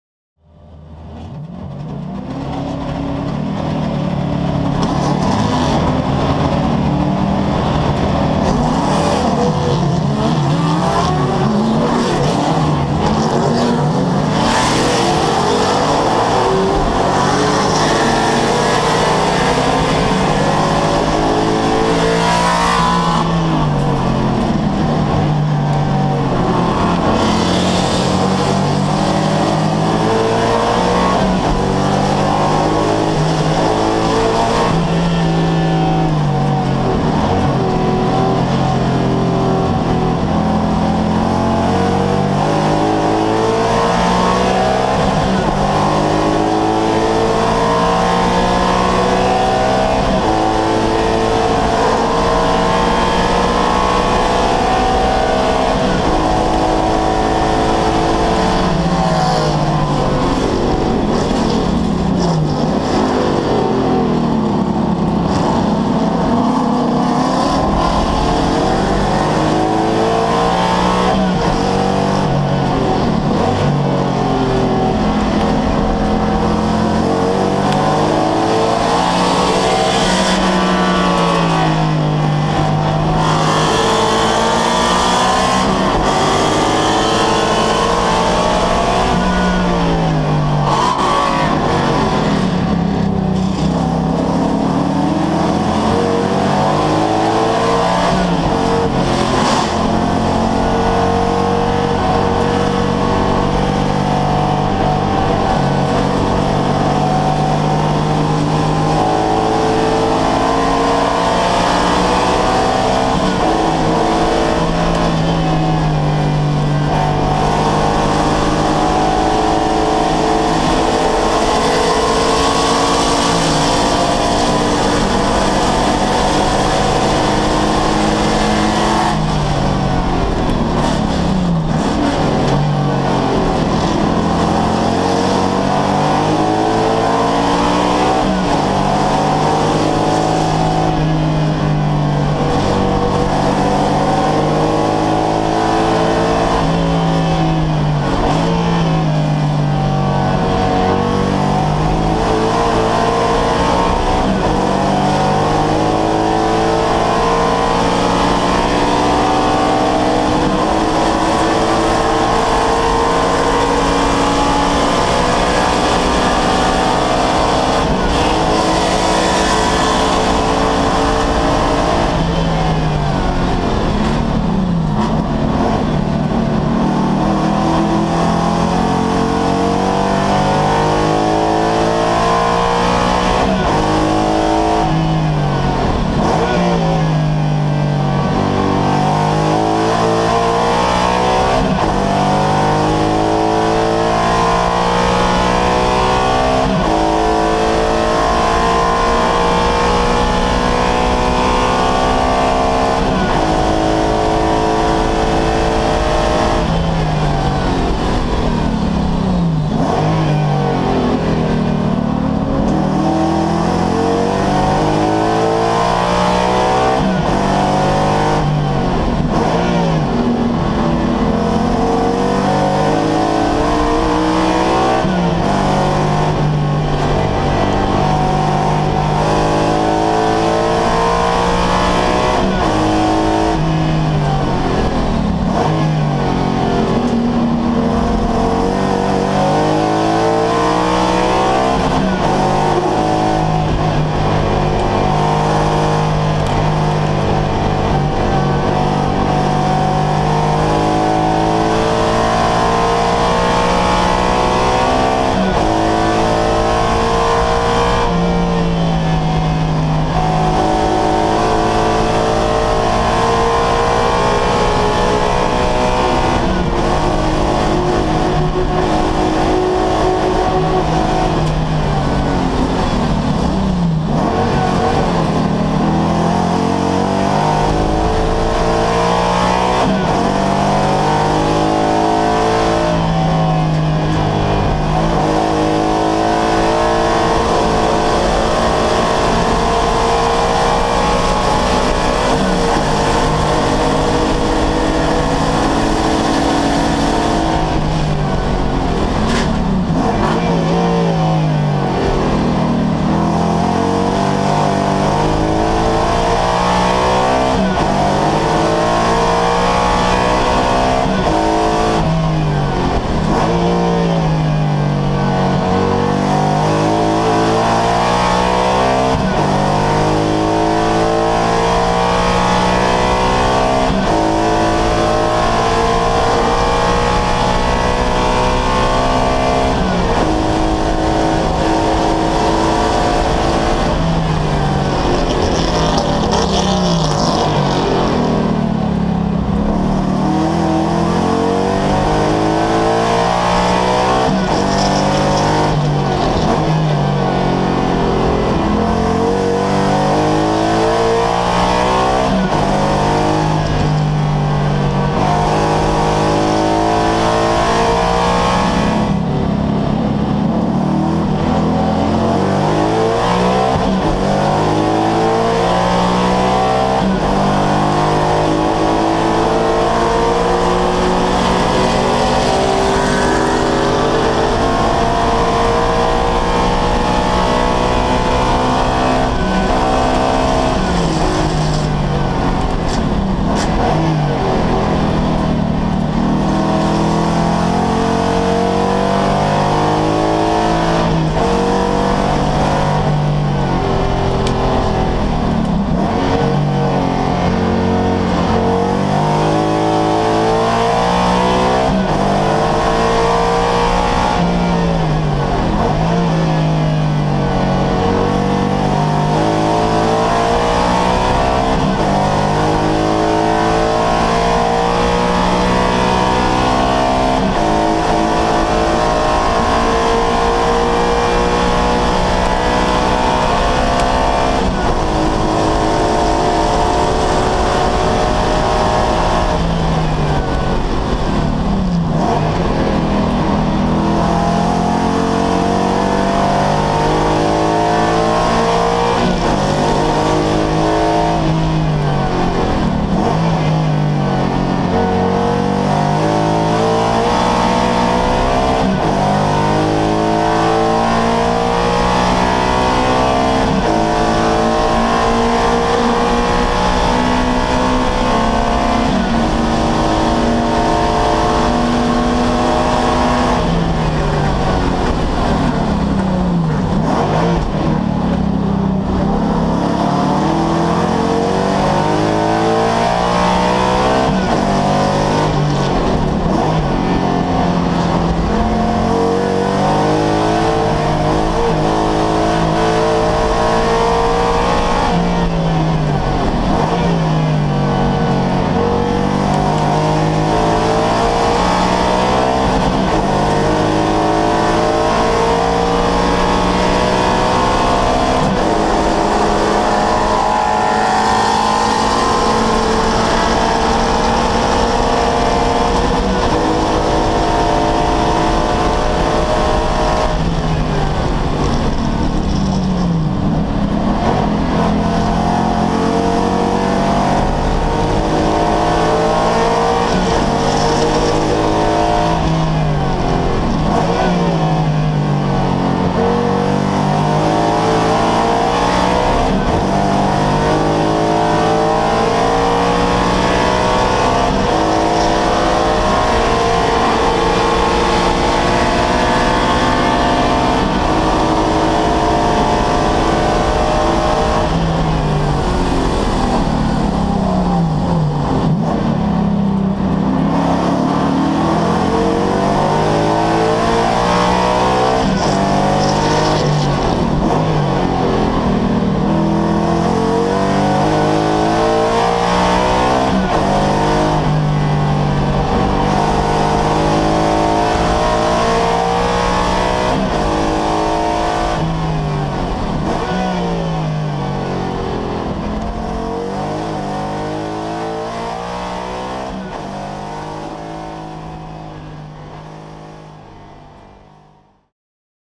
WRR TV: Perseverance Pays Off at Road America
Ride along onboard the #60 Ford Mustang Boss 302R of Jack Roush Jr. during the Pirelli World Challenge GTS class race at Road America in Elkhart Lake, Wisconsin on June 21st. After getting stuck behind slow traffic during the standing start, Jack battles his way through the pack to an eventual second place finish.

Winding Road Magazine is a free weekly online magazine written for the car enthusiast, with an intelligent, entertaining perspective.